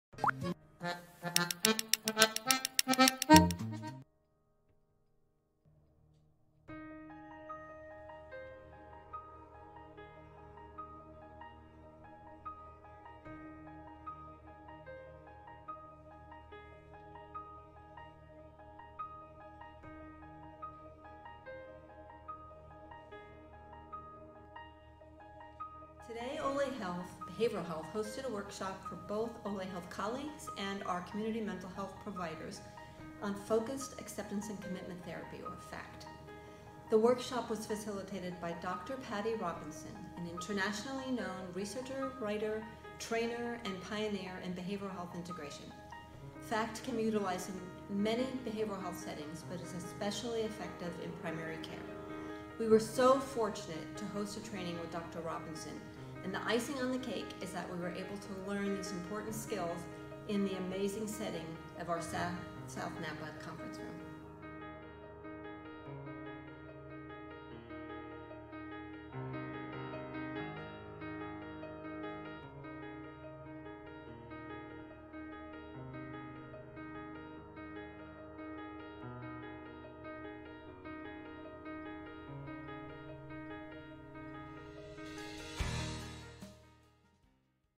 Monday Minute - Behavioral Health "FACT" Training
This all-day workshop included mental and behavioral health staff from throughout Napa County, hosted by OLE Health at the new Napa Valley Vintners South Napa Campus.